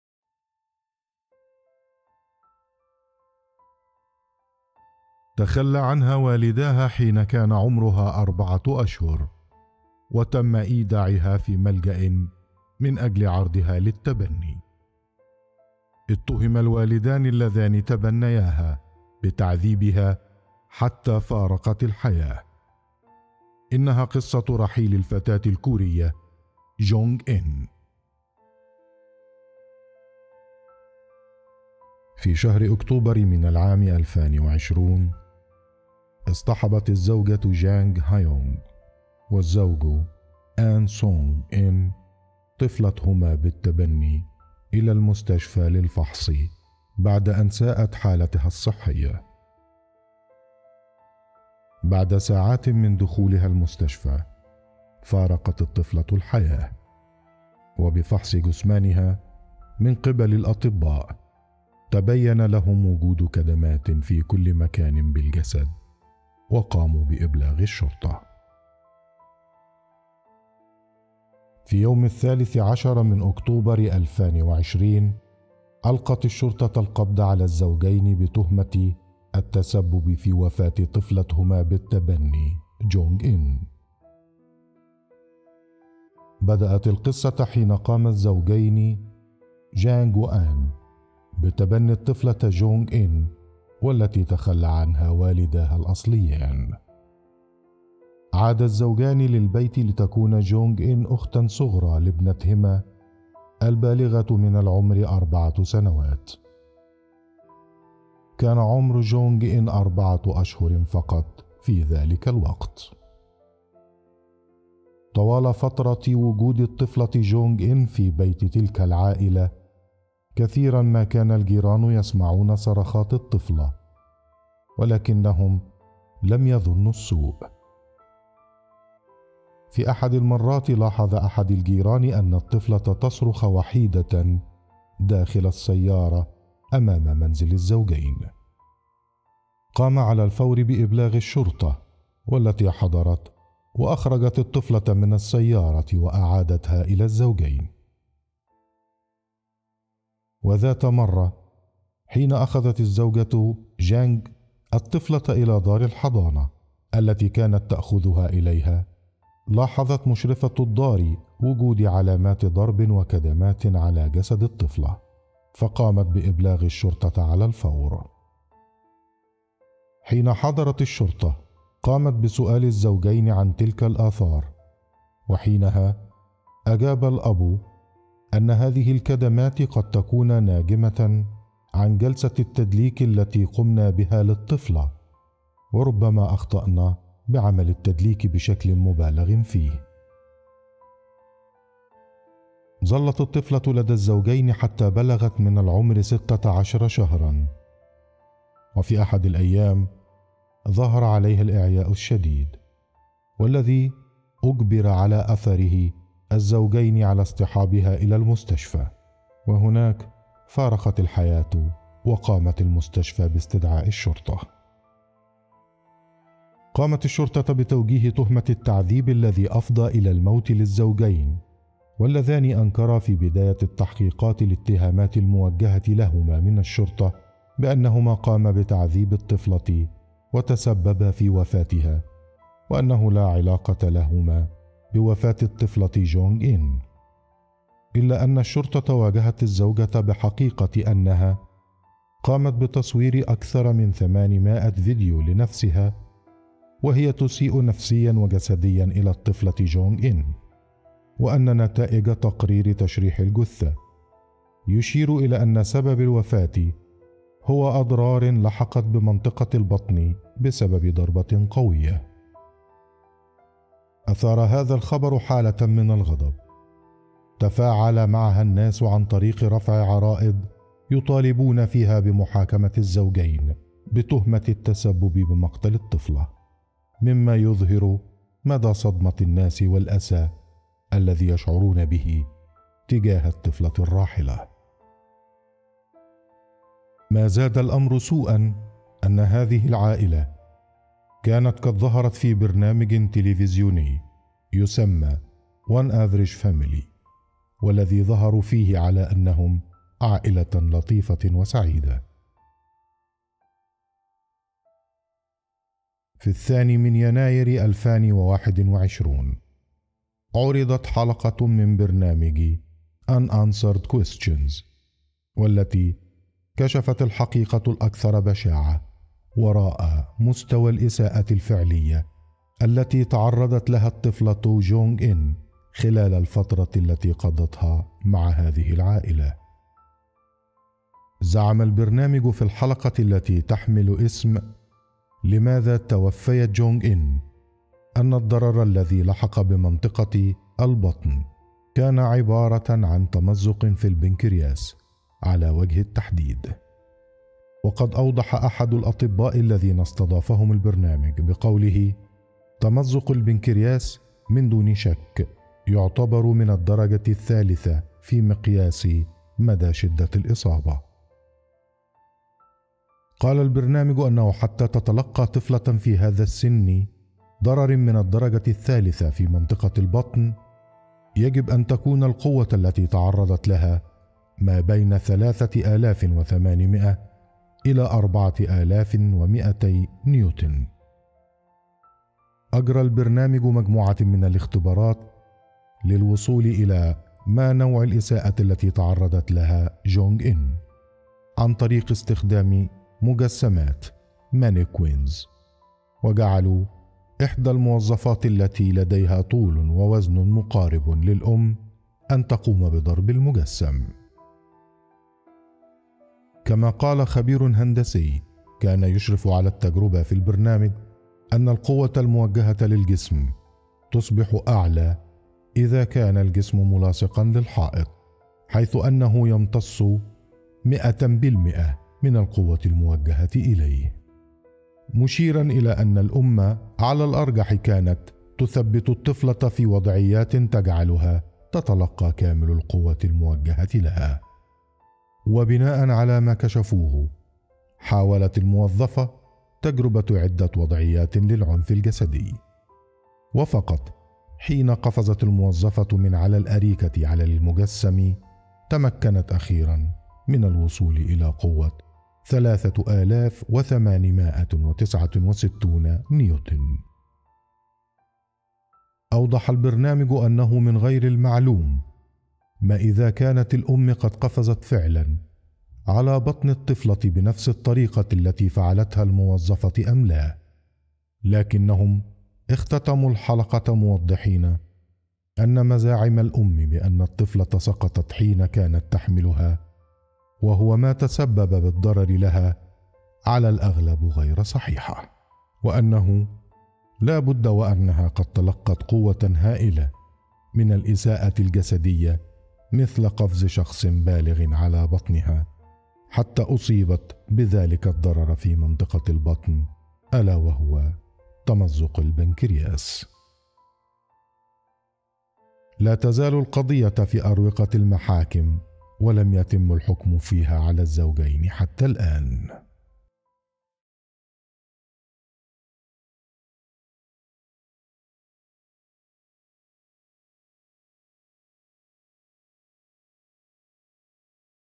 قضية الطفلة الكورية جونغ إن المتهم بقتلها والداها بالتبني
.ضع السماعات و استمتع بالفيديو
.كما اننا نرحب بمقترحاتكم عن ما تودون مشاهدته من فيديوهات